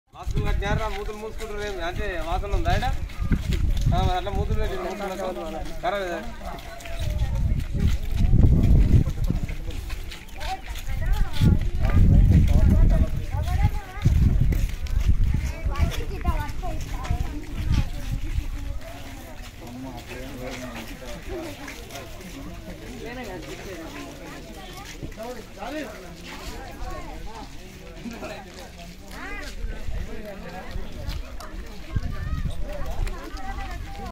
Jan Andolan campaign on COVID 19 - Rally and Booster dose distribution at Shankarapoor village
Jan Andolan campaign on COVID 19 - Unlock with precautions - Appropriate behaviour in the new normal Shankarapoor